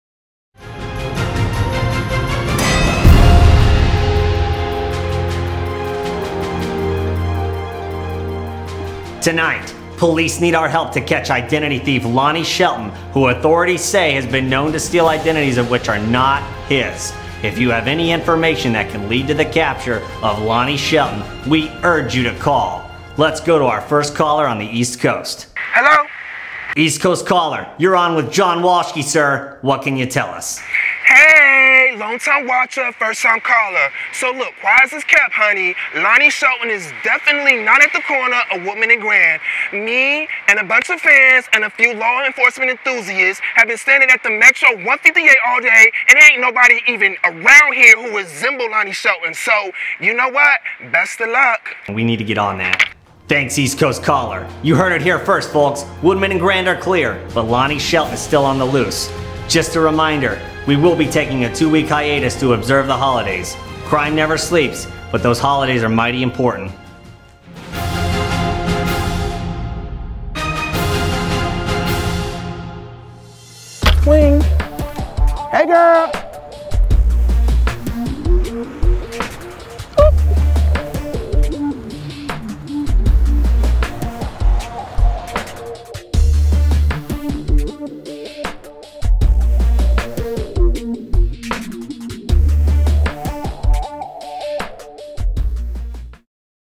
ETWshow - Help us catch Lonnie Shelton!
Help John Walshki and his team at PEOPLE THE UNITED STATES WOULD REALLY LIKE TO CATCH find identity thief Lonnie Shelton!